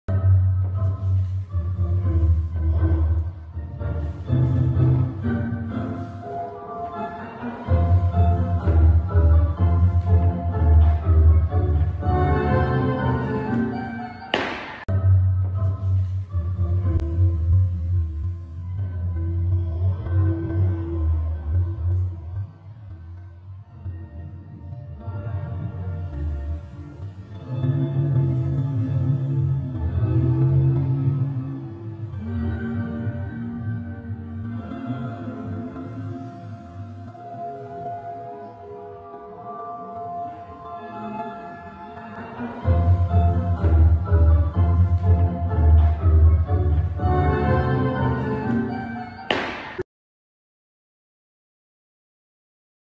Tango ~ "La cadena" - Nancy Fernando Tango
Tango lesson "La cadena" - Nancy Fernando Tango

Seguici / Chiedici l'amicizia!